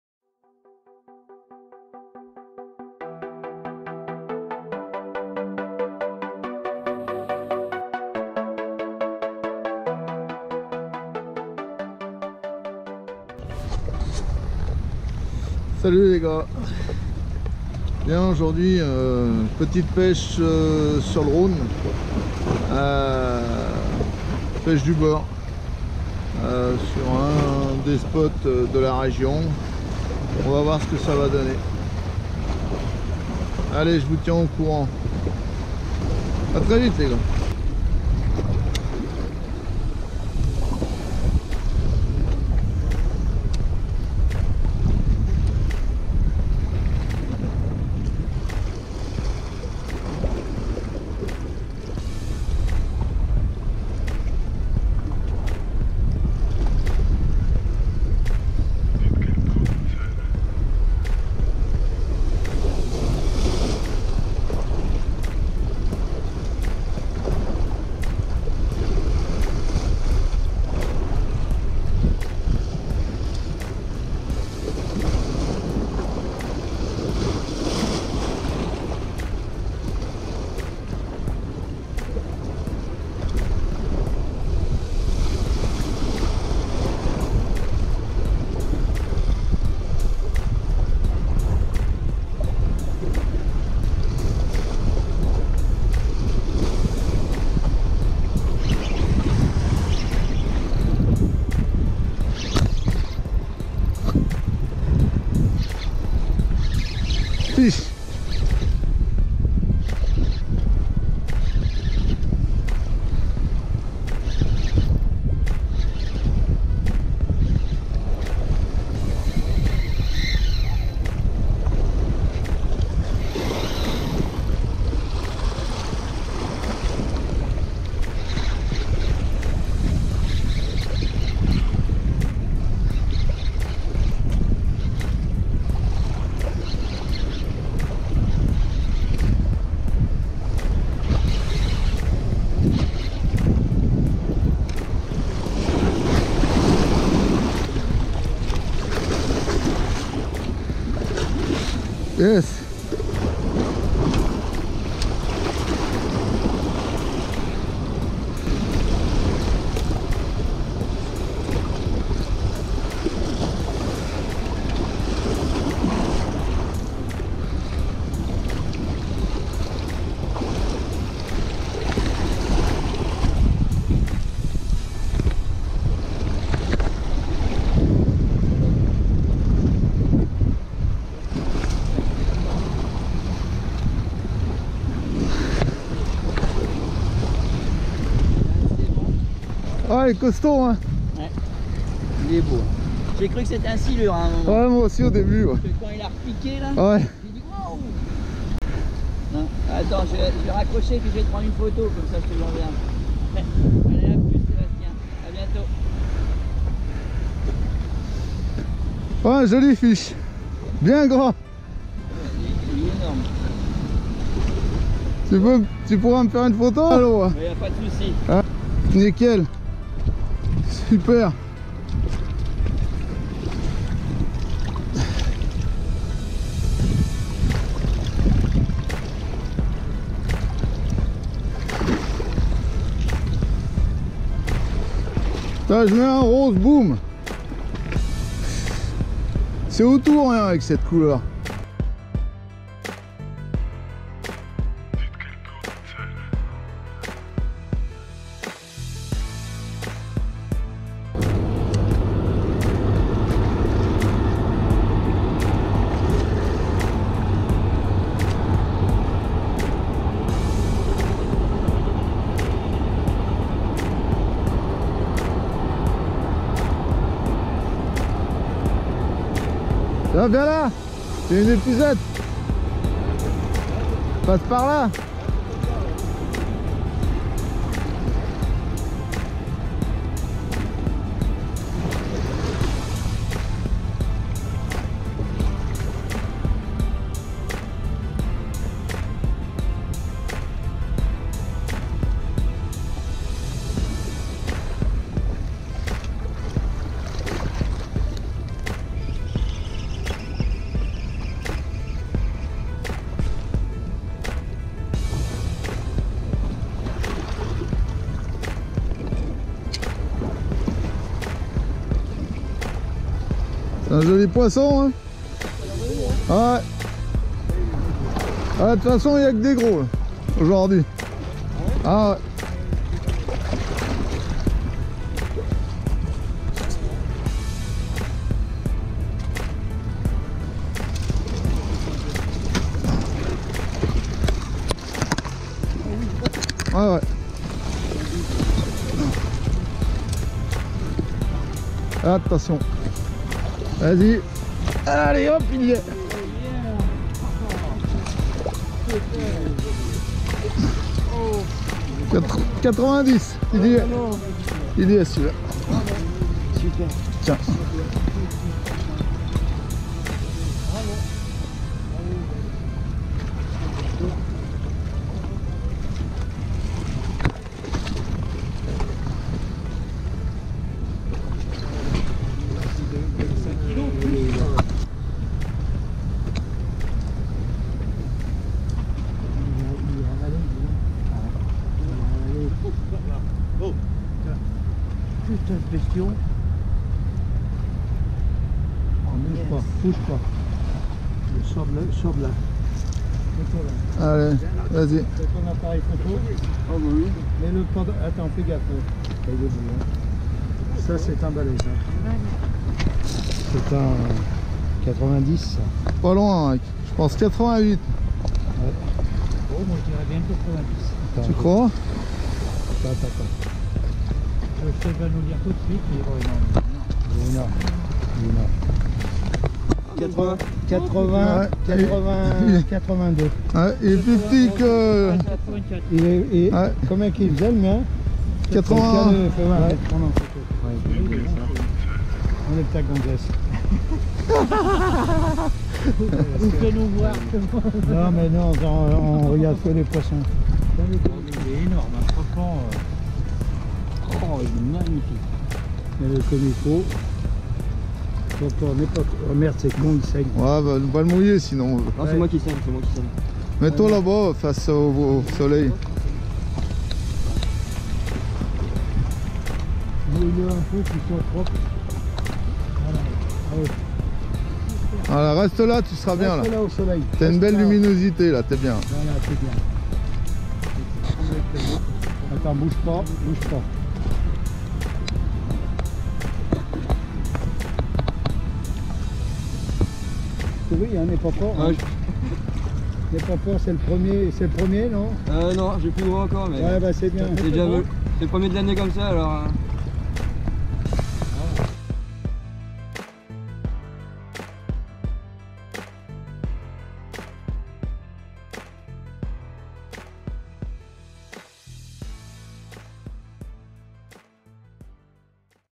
JOURNEE a GROS SANDRE sur le RHONE
une session du bord il y a quelques semaines avant la fermeture
peut de touche sur la journée mais 4 sandres tombent autour de moi ainsi que  moi même un vrai bonheur